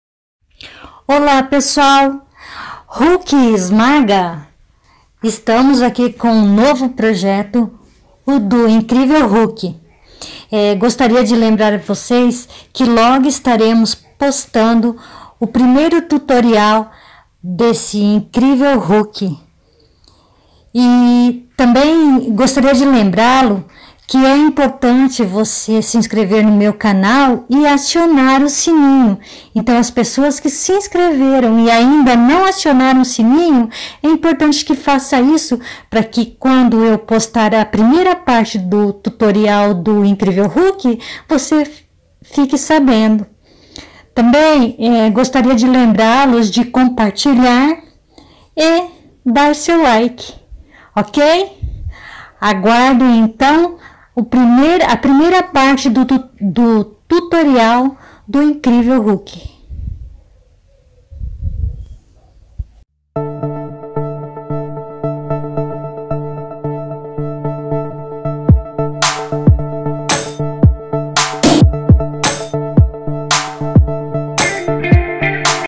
AMIGURUMI HULK
Amigurumi do incrível Hulk, personagem que se transformava em um monstro verde quando o personagem David Banner ficava com raiva, e atualmente personagem dos Vigadores.